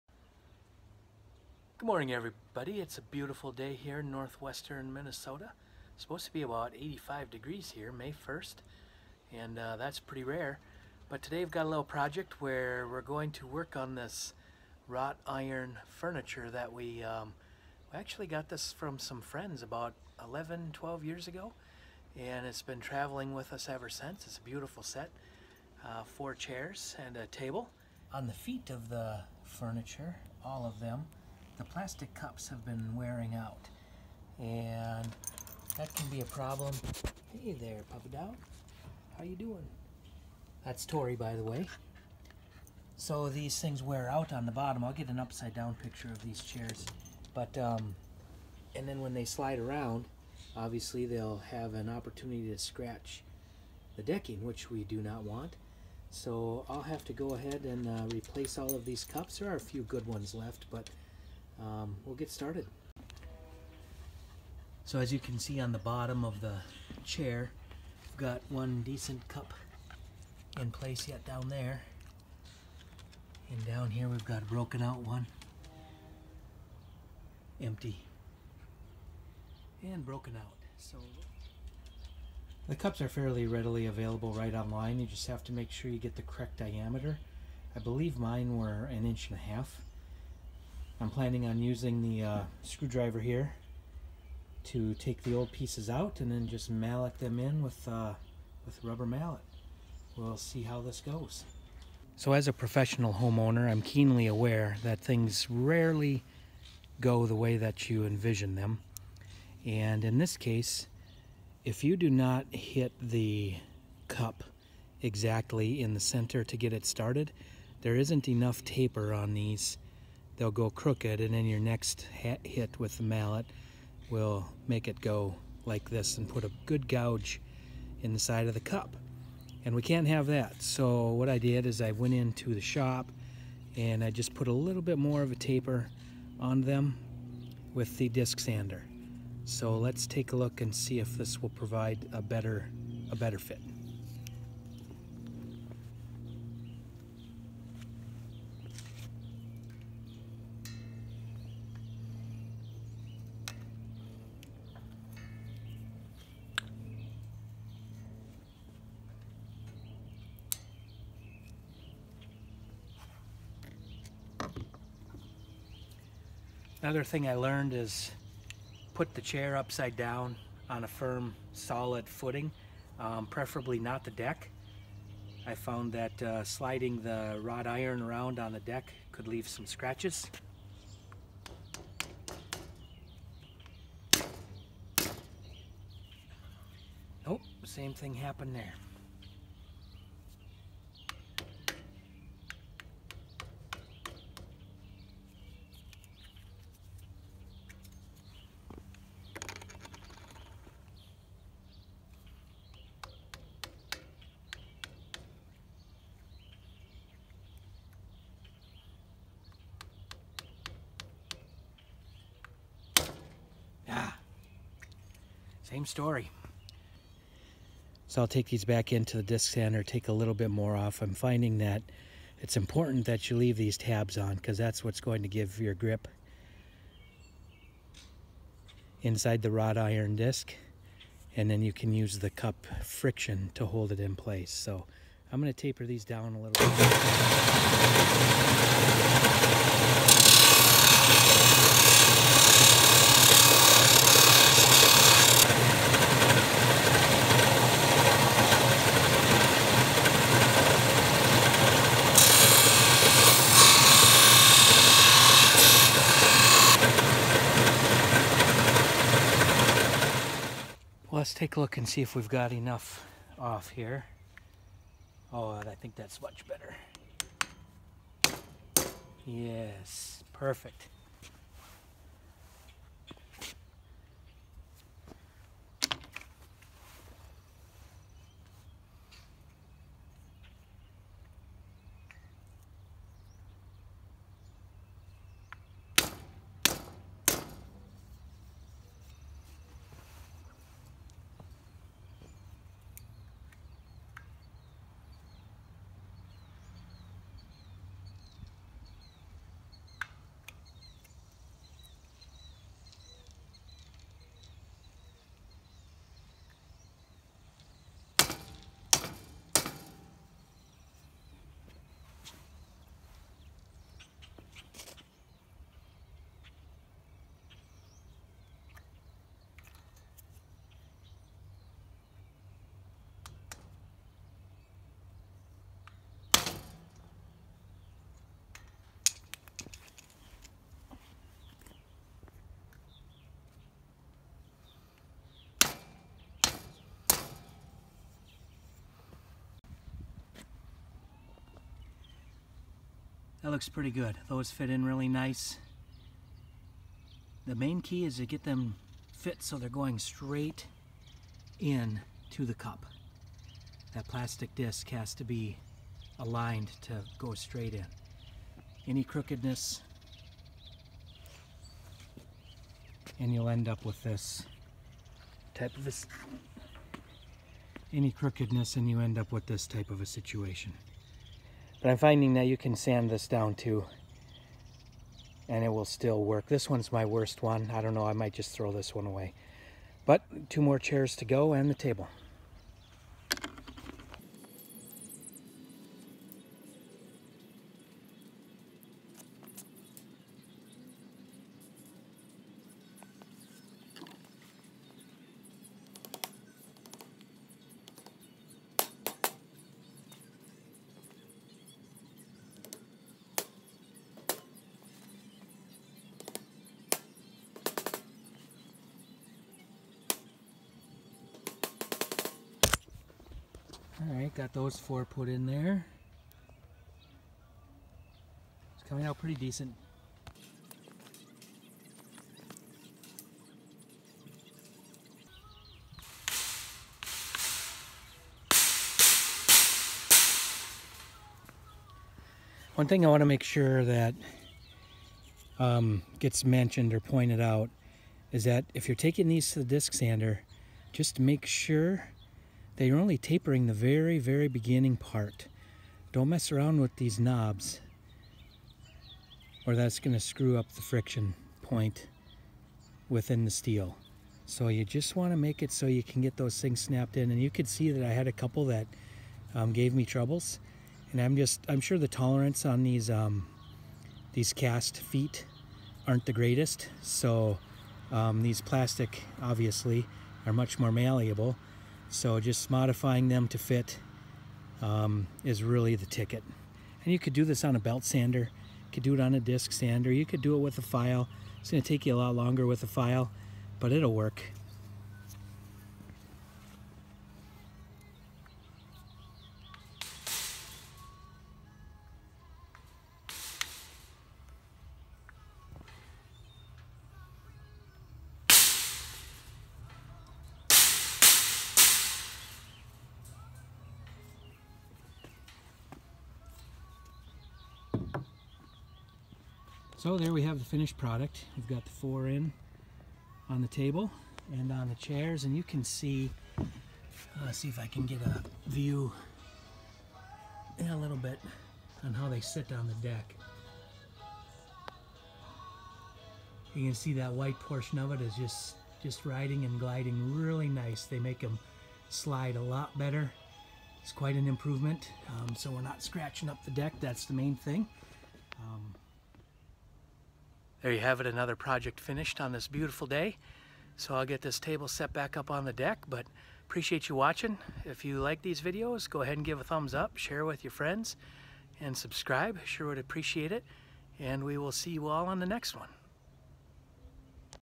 How To Replace The Feet Gliders On Wrought Iron Patio Furniture
In this video I show how to replace the plastic glides in the wrought iron feet cups of a patio set.